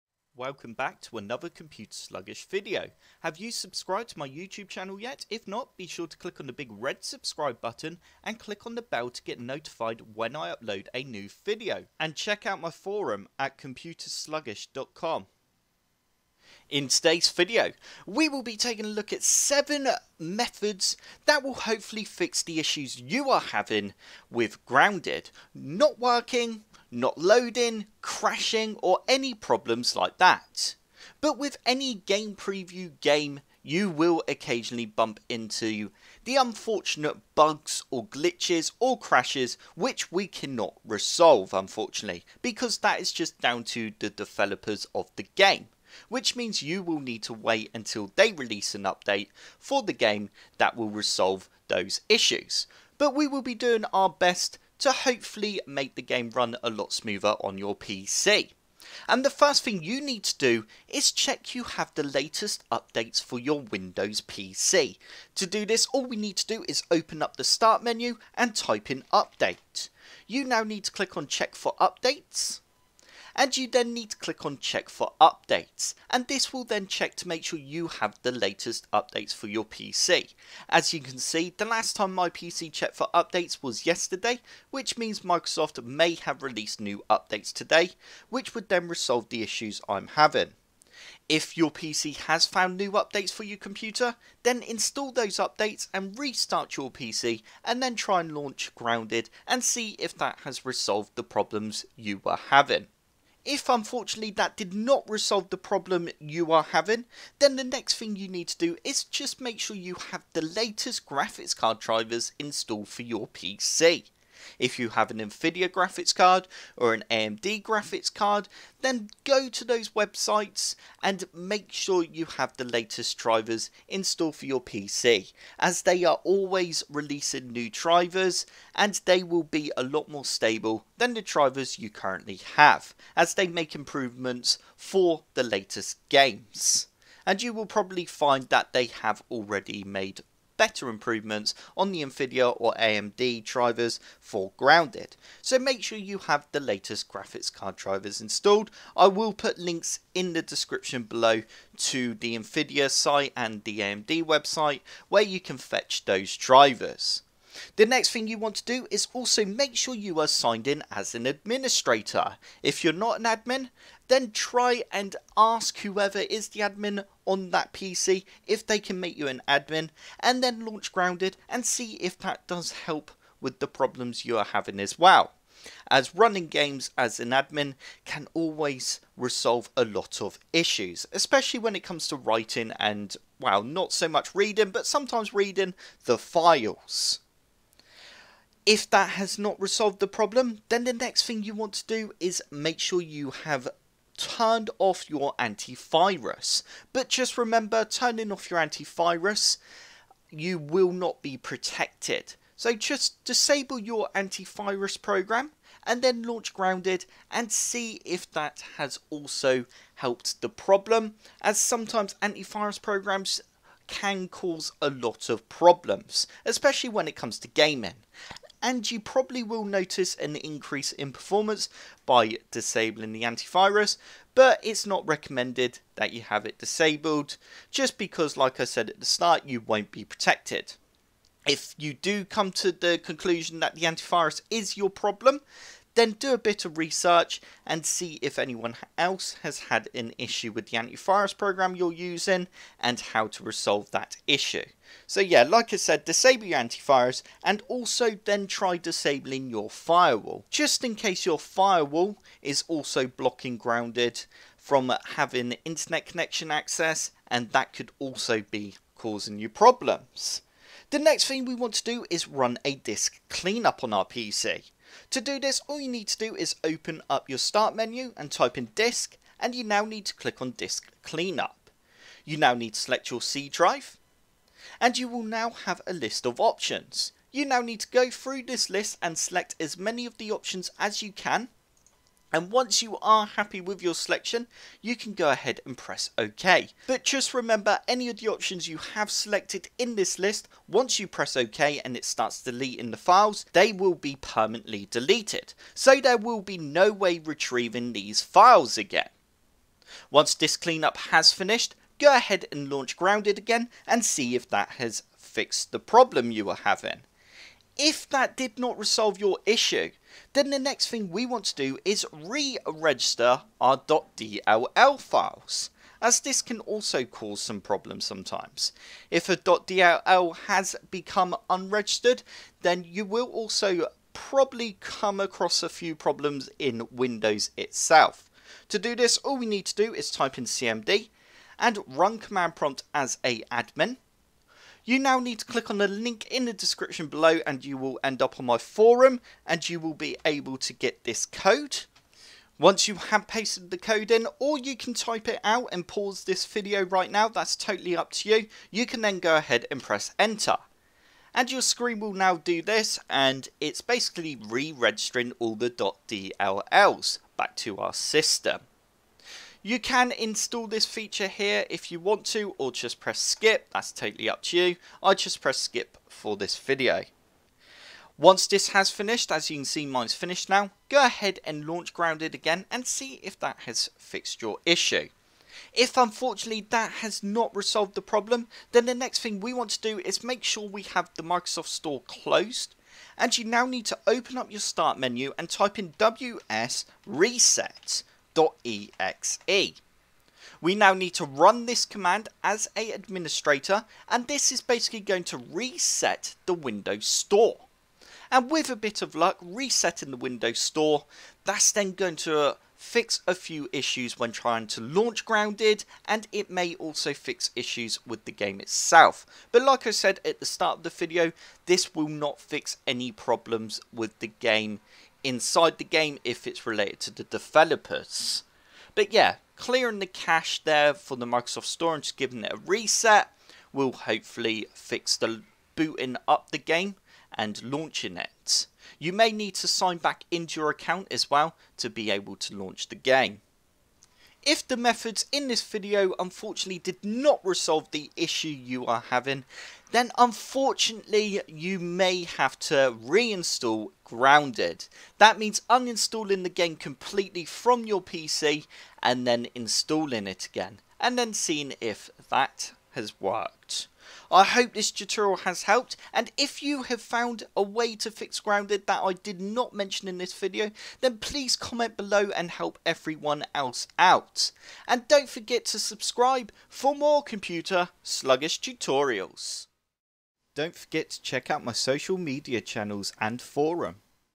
BEST 7 Methods To Fix Issues In Grounded On PC | Fix Crashes, Freezes & Loading Issues
In this grounded tutorial I will be showing you my top 7 recommended methods to fix most issues you may be encountering in this new game on your Windows 10 PC or laptop. If you have been having problems with grounded not working properly such as; the game crashing, freezing and not loading, then don't panic because there are a few things you can try that will hopefully fix the errors you are having.

Just remember this game is still in early access so if you are getting problems such as dev errors and not a Windows 10 issue then it could simply be that you need to wait for a new update or  patch to install.

The code to re register the DLL's can be found

For step by step tutorials, or if you would like to request any tutorials or need any PC help then check out my